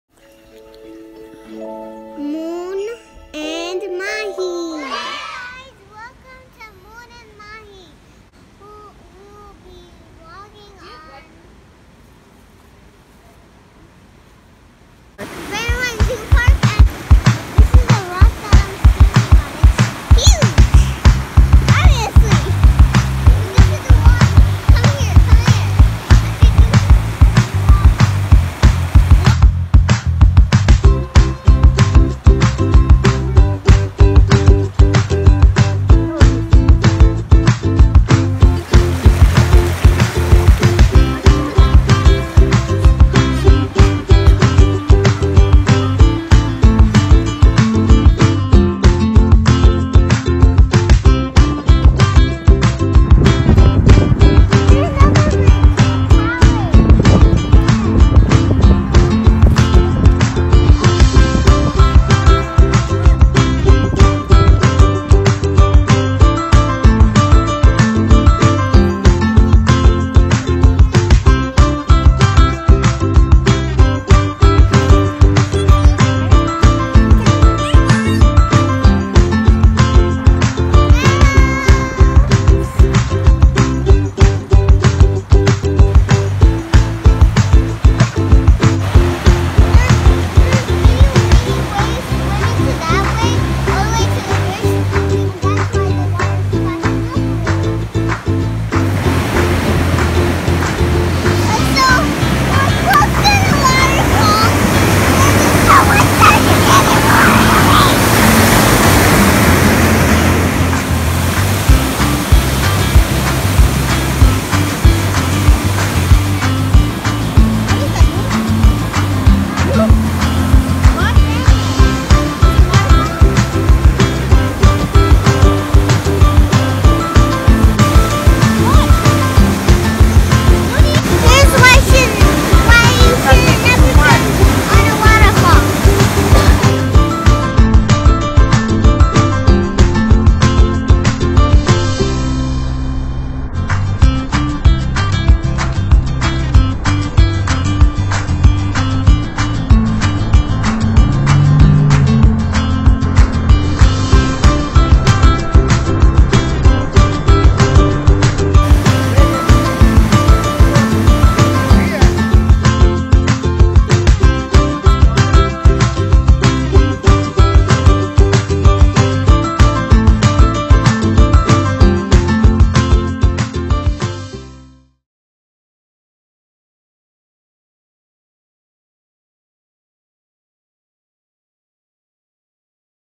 Vlog- Going to Brandywine Zoo Park!
There's mini waterfalls, rivers, pretty bridges, hills, historical sites and more. Although this place would be very beautiful in the summer,
it's pretty breathtaking even in the winter.

It's a New Year so Mun and Mahi decided to go on this mini adventure and have some fresh air as well.